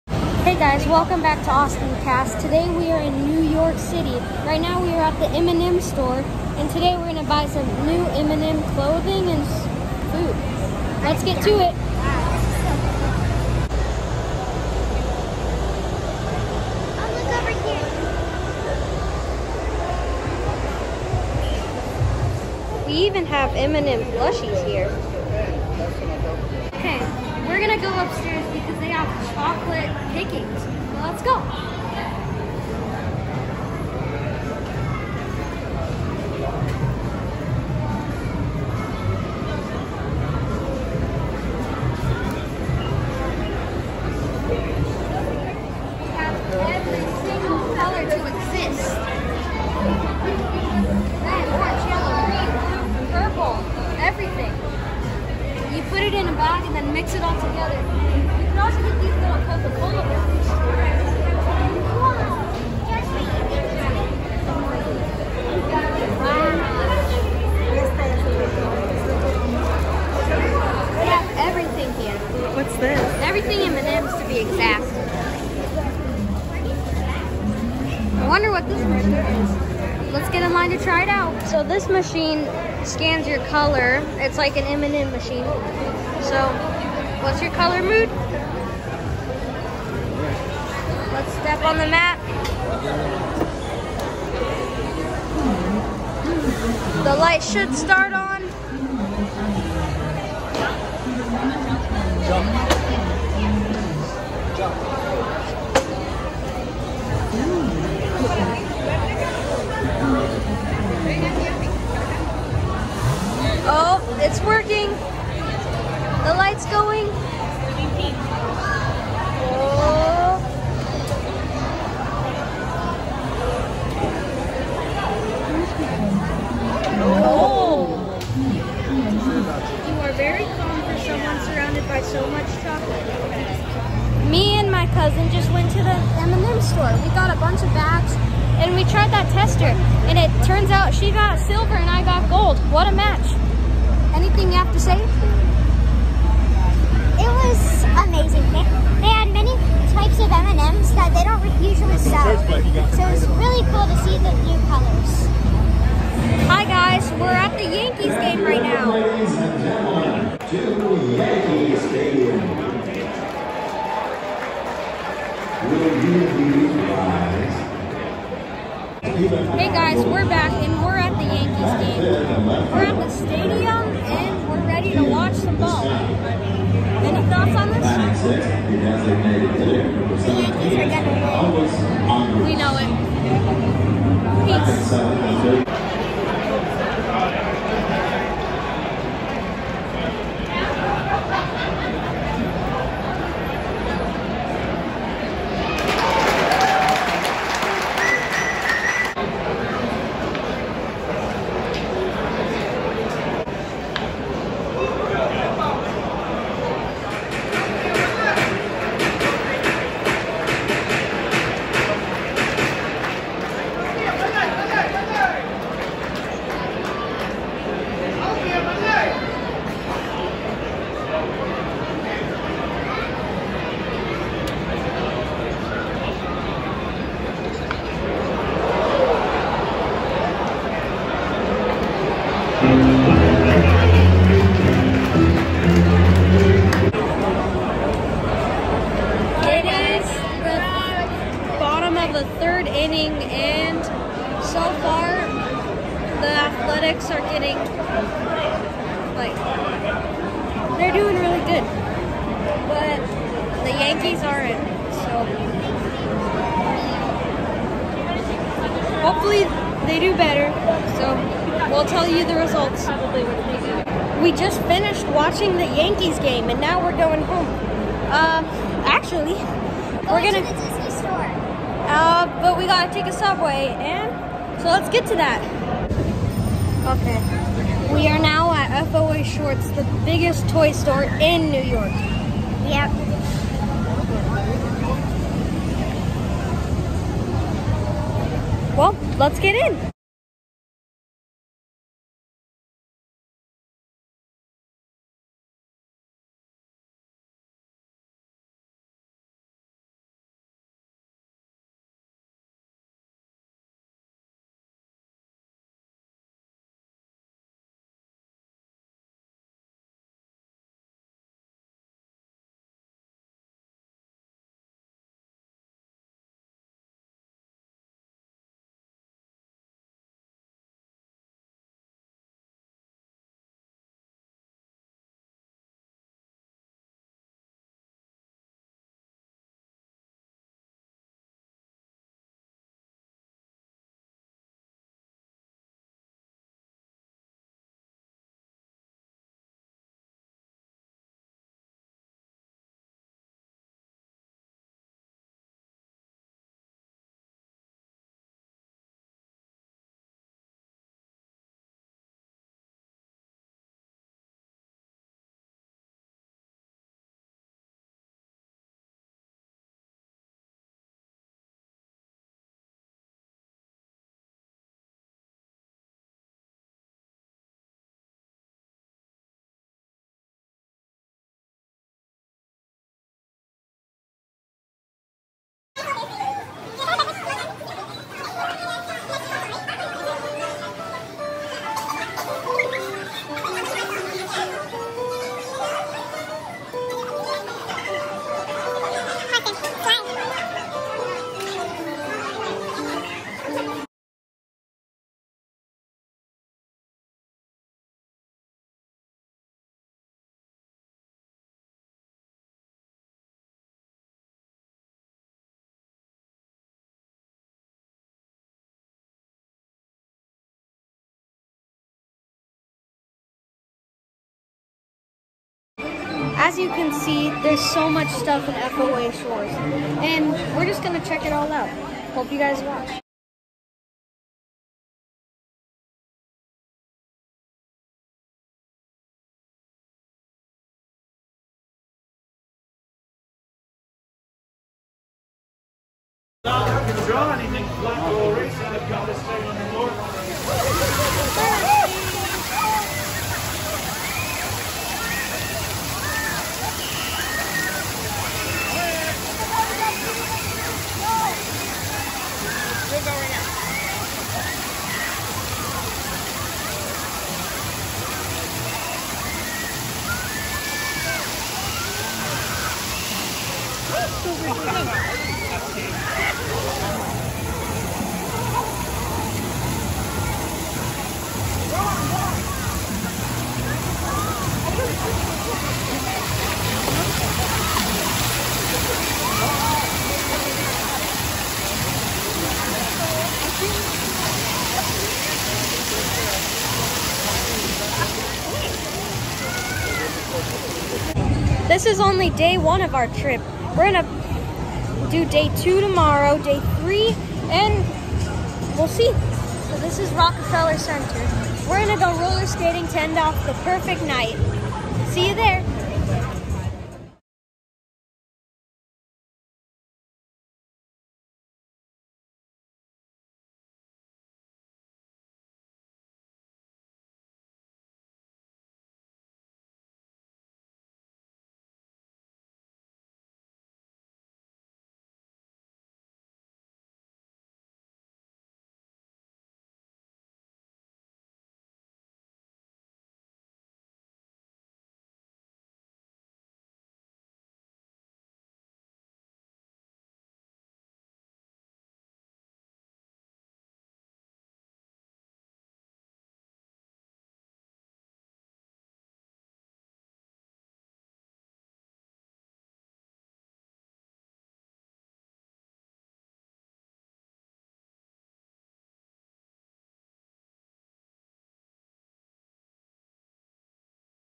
My New York Trip (Day 1) CANDY EVERYWHERE 🍩🧁 toys and baseball
Se will be doing a sequence of travel vids and we will still be uploading constant LEGO videos and gaming videos might be soon to come….

AustinCast starring -
  Austin
 Berkeley